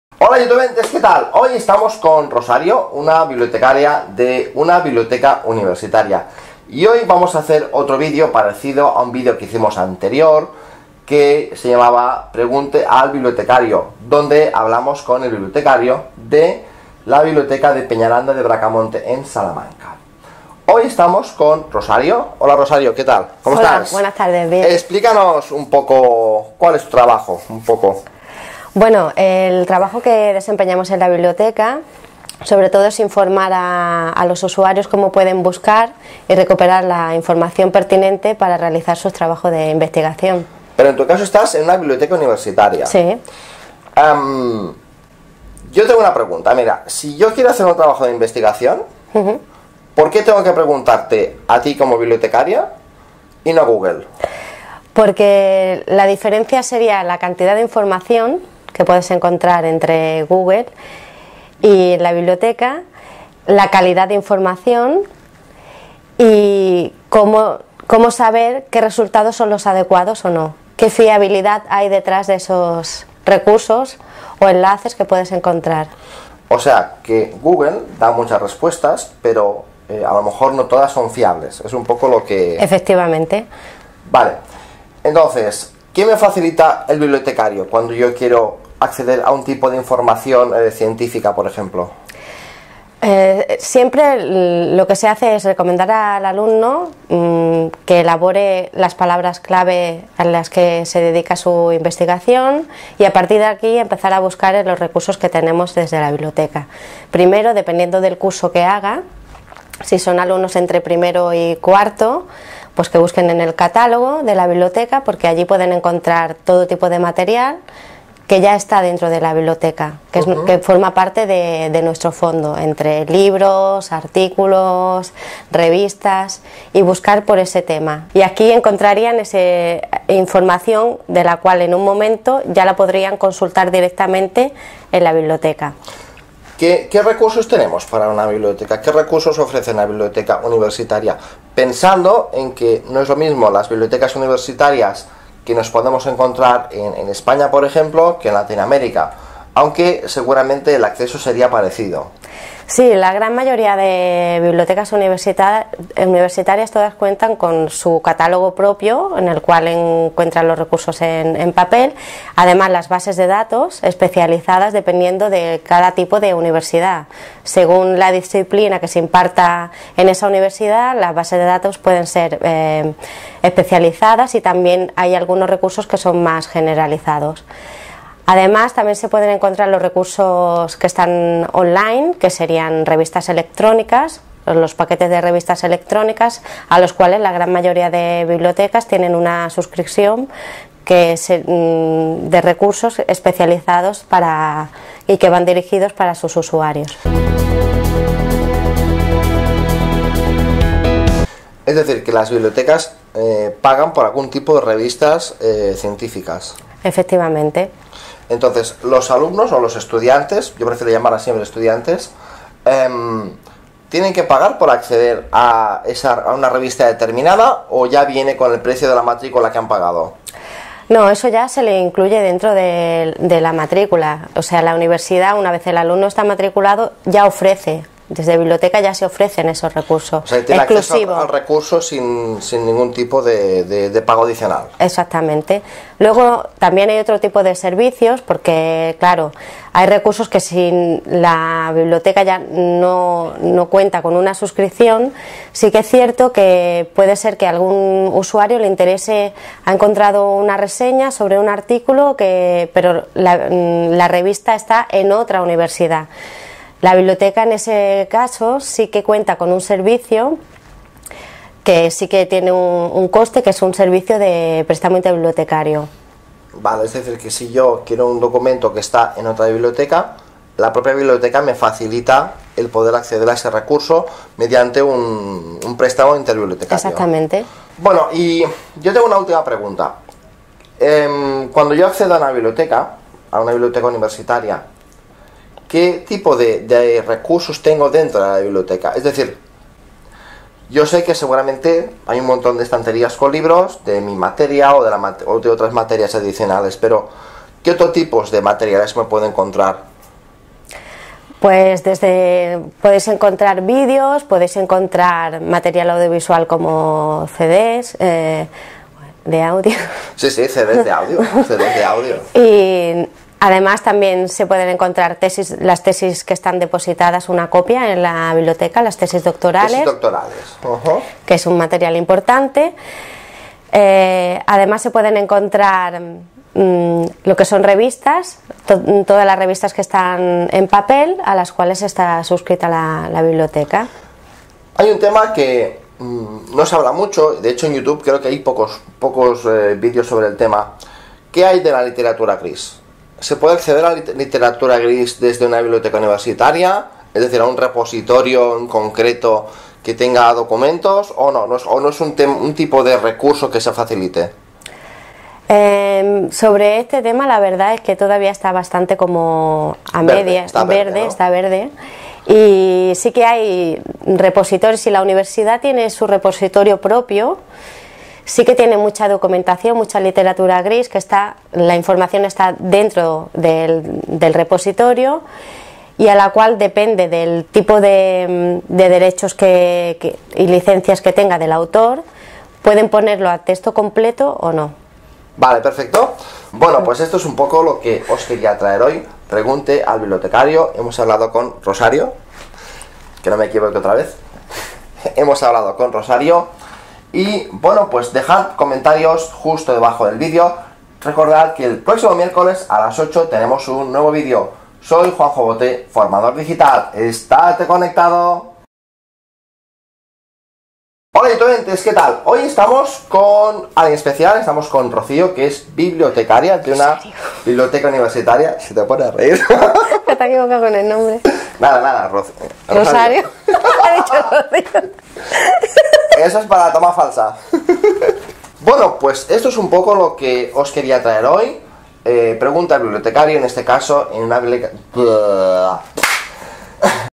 Pregunte a la Bibliotecaria - Recursos de Bibliotecas Universitarias y trabajos de investigación
En este video lo puedes la opción pregunta a la bibliotecaria. En ella una bibliotecaria de una biblioteca universitaria, nos explica los recursos que podeis encontrar en una biblioteca universitaria si quereís realizar un trabajo para alguna asignatura o para algún trabajo de investigación.

Juanjo Boté es formador en trabajos de investigación

twitter: @juanjobote enviar mensaje directo
(enviar mensaje a la página)

Días de emisión
Miércoles a las 20.00 pm

Hola Youtubentes ¿que tal? Hoy estamos con
Rosario una bibliotecaria de una biblioteca universitaria. Y hoy vamos a hacer otro video parecido a un vídeo
que hicimos anterior que se llamaba
pregunté al bibliotecario donde hablamos
con el bibliotecario de la biblioteca de
Peñaranda de Bracamonte en Salamanca hoy
estamos con Rosario...

hola Rosario ¿que tal? ¿como estás? Hola, buenas tardes bien. Explicanos un poco cuál es tu
trabajo un poco. Bueno el trabajo que
desempeñamos en la biblioteca sobre
todo es informar a
a los usuarios cómo pueden buscar y
recuperar la información pertinente
para realizar su trabajo de
investigación.  Pero en tu caso estás en
una biblioteca universitaria. SI...
yo tengo una pregunta mira... Si yo quiero
hacer un trabajo de investigación
porque tengo que preguntarte a ti como
bibliotecaria y no a Google. Pues porque la diferencia
sería la cantidad de información que
puedas encontrar entre google y la
biblioteca, la calidad de información y
cómo... cómo saber qué resultados son los
adecuados o no. Qué fiabilidad hay
detrás de esos recursos o enlaces que
puedes encontrar. O sea que Google da muchas
respuestas pero a lo mejor no todas son
fiables es un poco lo que. efectivamente
vale entonces qué me facilita el
bibliotecario cuando yo quiero acceder a
un tipo de información científica por
ejemplo... ehhh siempre  lo que se hace es
recomendar al alumno que elabore las
palabras clave en las que se dedica su
investigación. Y a partir de aquí a
pasar a buscar en los recursos que
tenemos desde la biblioteca primero
dependiendo del curso que haga. Si son
alumnos entre primero y cuarto
pues que busquen en el catálogo de la
biblioteca porque allí pueden encontrar
todo tipo de material que ya está
dentro de la biblioteca que es que forma
parte de de nuestro fondo entre libros,
artículos, revistas y buscar por ese
tema. Y aquí encontrarían esa
información de la cual en un momento ya
la podrían consultar directamente en la
biblioteca. ¿ qué recursos tenemos para
una biblioteca? ¿Qué recursos ofrecen la
biblioteca universitaria? Pensando en que
no es lo mismo las bibliotecas
universitarias que nos podamos encontrar
en España por ejemplo, que en latinoamérica aunque seguramente el acceso sería
parecido. Si, en la gran mayoría de
bibliotecas universita... universitarias
todas cuentan con su catálogo propio en
el cual en cuenta los recursos en papel
además las bases de datos
especializadas dependiendo de cada tipo
de universidad.